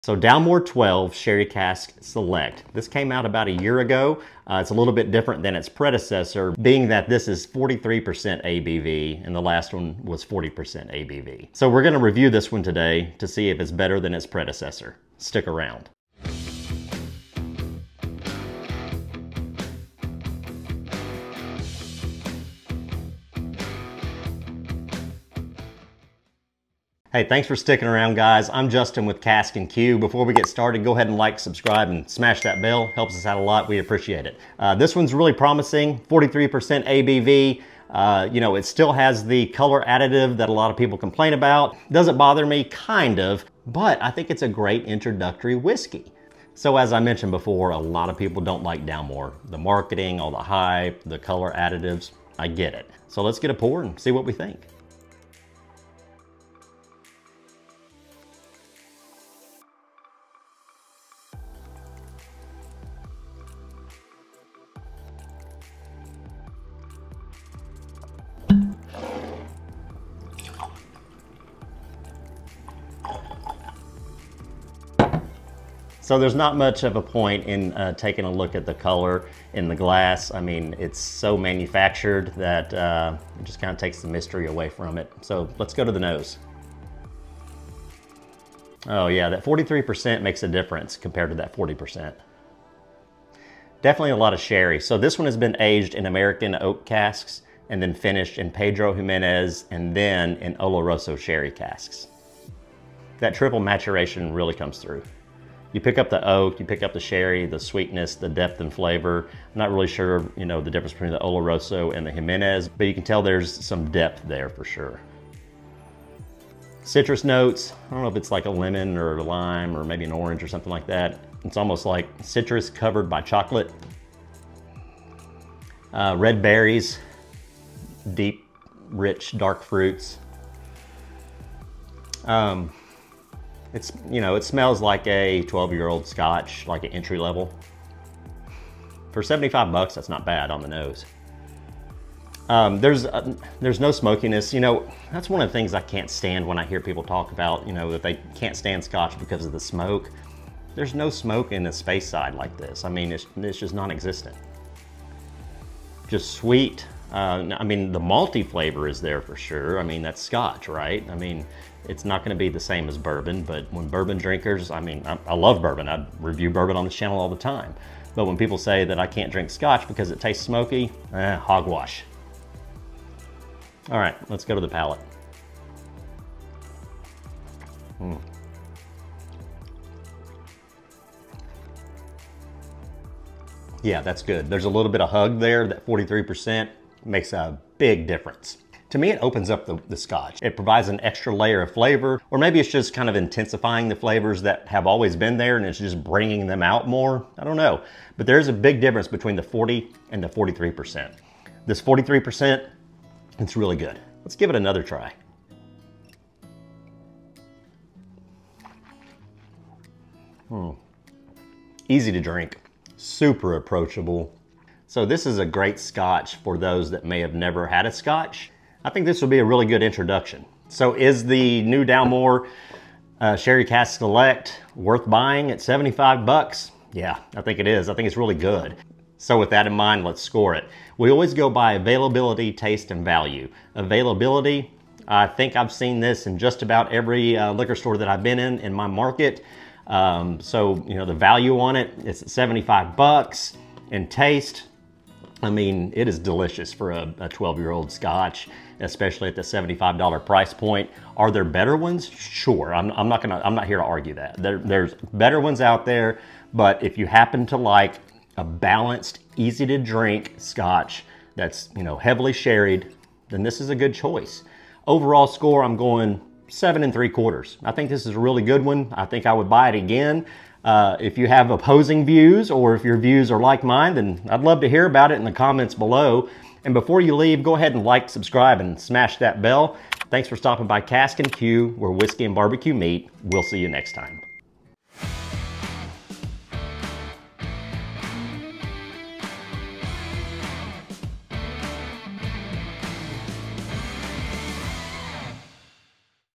Dalmore 12 | Sherry Cask Select | Scotch
Dalmore 12 | Sherry Cask Select | Scotch Whisky Review

Uncorking the Dalmore 12 Year Old Sherry Cask Select on Casks-n-Que! This Highland single malt gets a special finishing touch in unique sherry casks, creating a decadent flavor profile. We'll be diving deep into its aroma, taste, and finish - is it worth the price tag?

00:00 Introduction
00:27 Like, Subscribe & Click the Bell
00:37 Facts
00:48 Bad things about Dalmore
01:12 Aroma & Tasting
03:33 Worth the money?